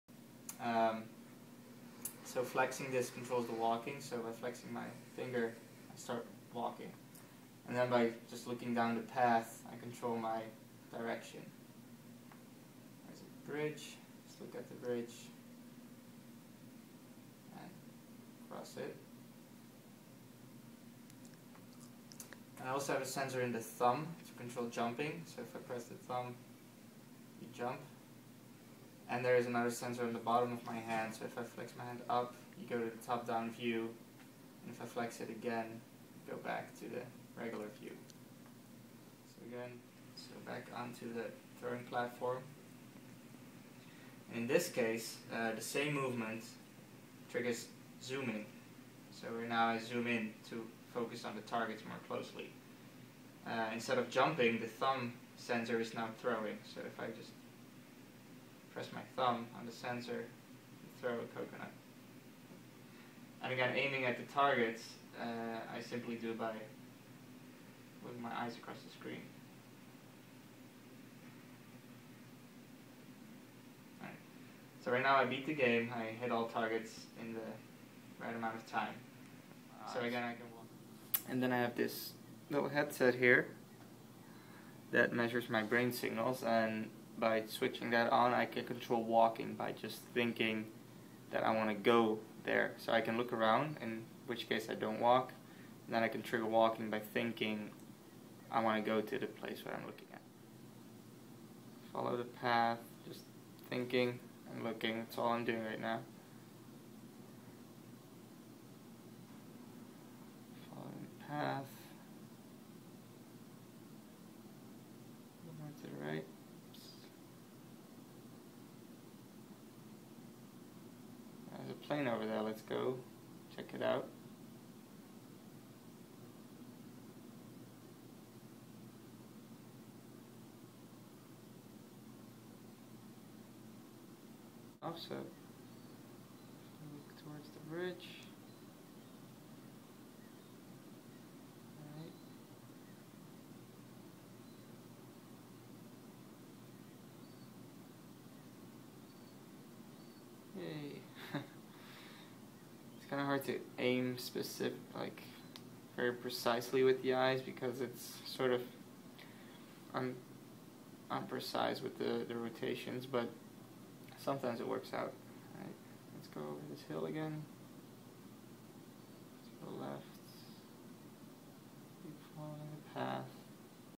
Gaming using eye-tracking, a self-built data-glove, and an Emotiv EEG headset
This is a video I made in 2011 to demonstrate the two systems I had been working on:

(1) An Eye-tracking + Data-glove interface, which allows users to explore and interact with a virtual environment using their eyes and one hand only.

(2) An Eye-tracking + EEG headset interface, which allows users to explore and interact with a virtual environment using their eyes and mind alone (handsfree).

Both systems were showing promising results